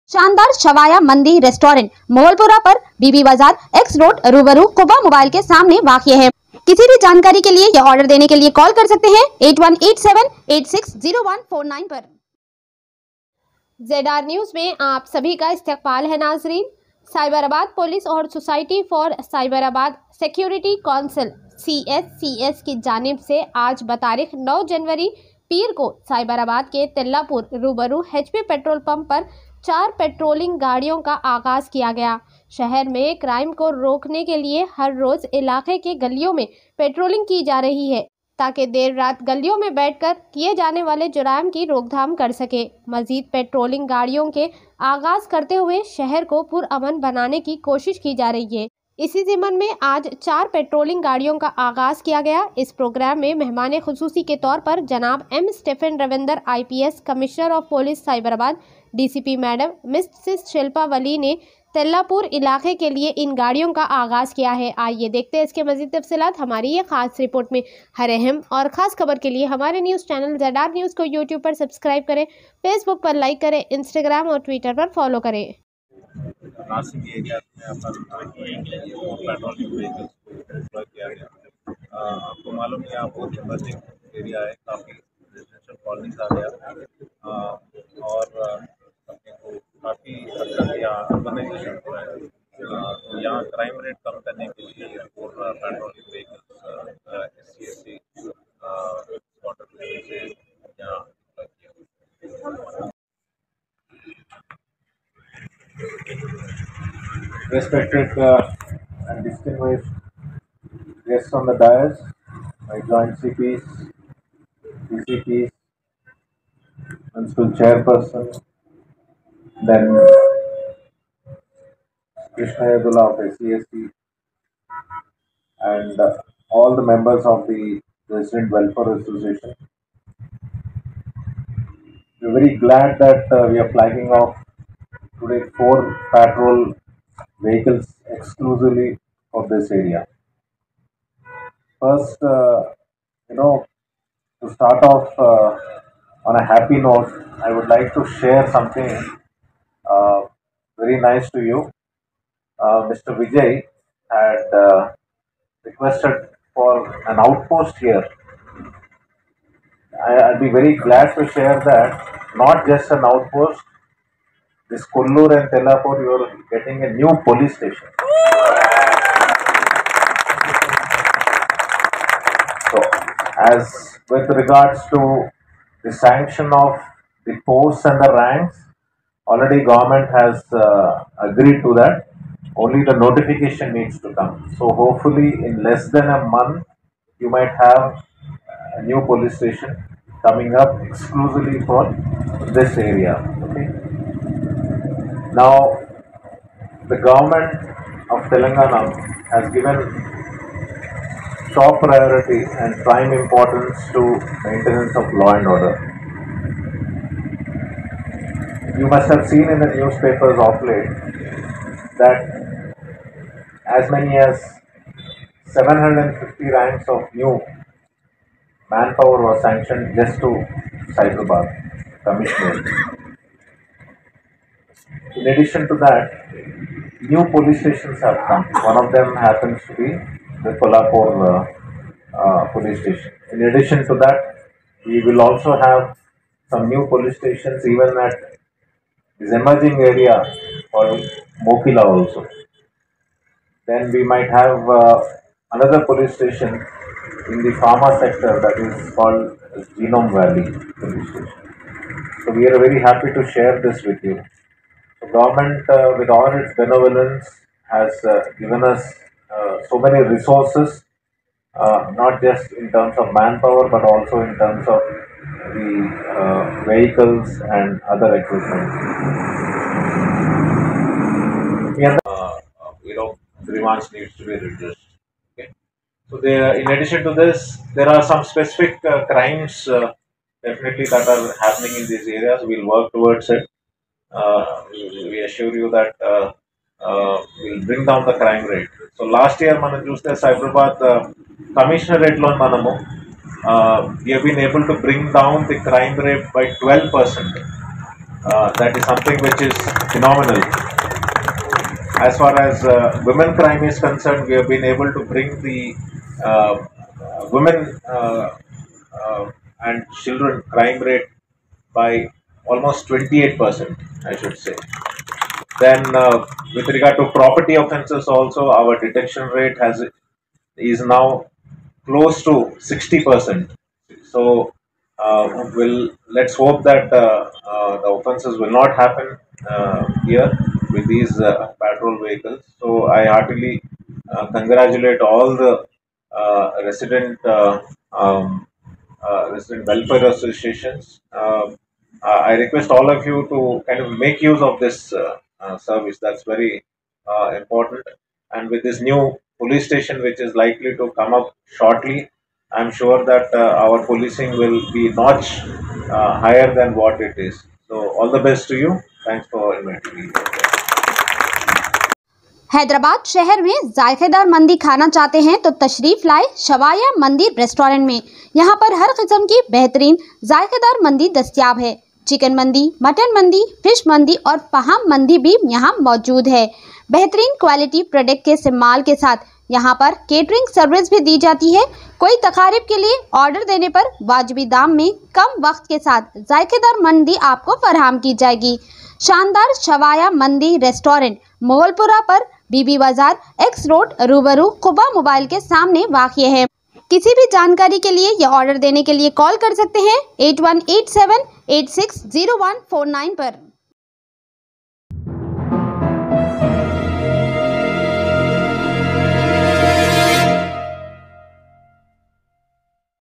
Launch Of Patrol Vehicles For Tellapur Cyberabad | Good News More New Police Station Will Getting
Cyberabad police Or Society for cyberabad security council Ke Janib Se Ajj 4 Patrol Vehicles launch Kiye Gaye Jiske CHIEF Guest Mr.M. Stephen Raveendra Ips Commissioner of police cyberabad or DCP Madam Ms.Shilpavalli Ne Tella pura area Ke Liye Launch Kiya ! @zrnews1